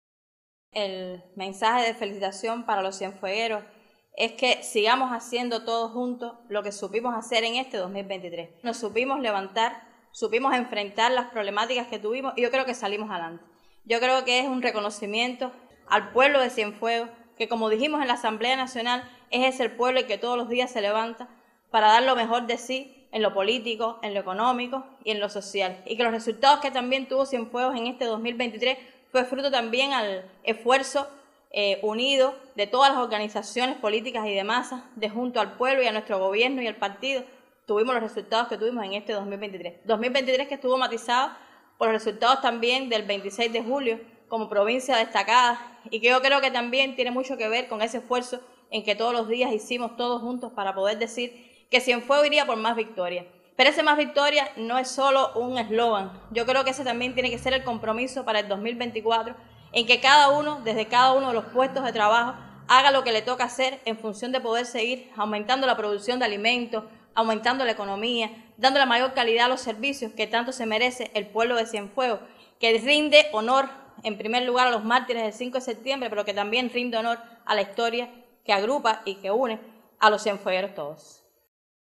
Mensaje de Primera Secretaria del Partido a pueblo de Cienfuegos
Marydé Fernández López, Miembro del Comité Central del Partido y su Primera Secretaria en la provincia de Cienfuegos envía un mensaje de salutación al pueblo a propósito de un nuevo aniversario del triunfo de la Revolución Cubana.